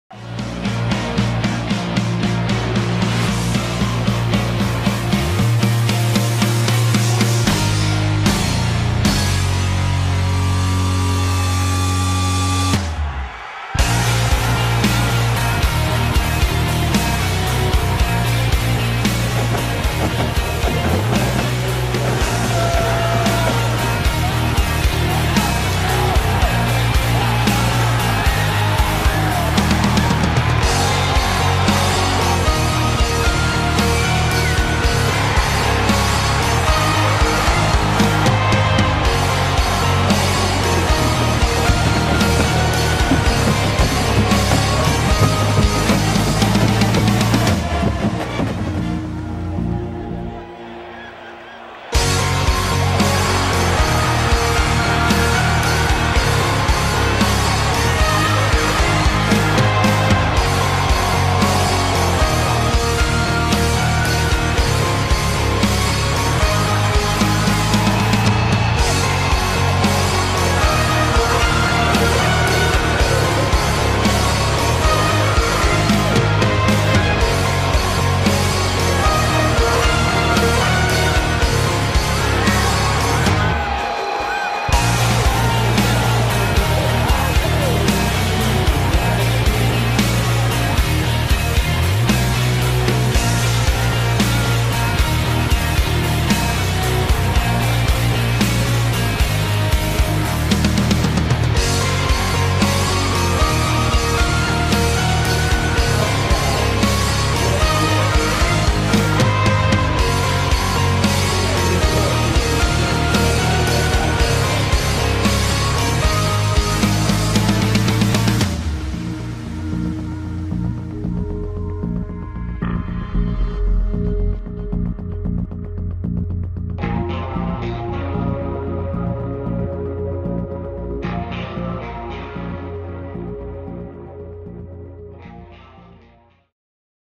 Throwback Thursday - Texas State vs SMU 2008
Take a look back at the 2008 matchup between Texas State (FCS at the time) and Southern Methodist.  The Bobcats fell to the Mustangs by a score of 47-36, but Texas State finished the season strong winning the Southland Conference.